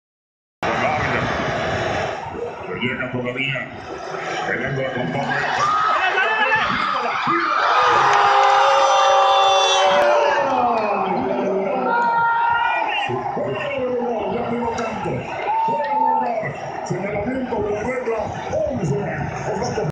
1645 Reacciones en Monterrey al Gol anulado del Chucky Lozano. México vs Arabia Saudita.
30 noviembre, 2022 Si te gusta el ajedrez revisa mis otros canales Si les gusta el ajedrez pueden pasar por mi otro canal